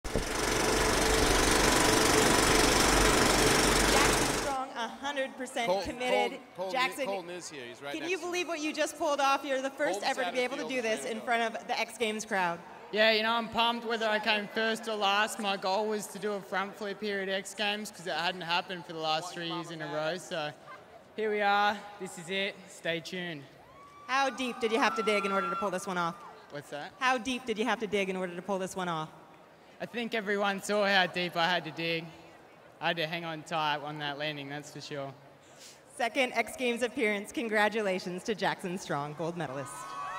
X Games 17: Jackson Strong Interview
Interview with X Games 17 Moto X Best Trick winner Jackson Strong.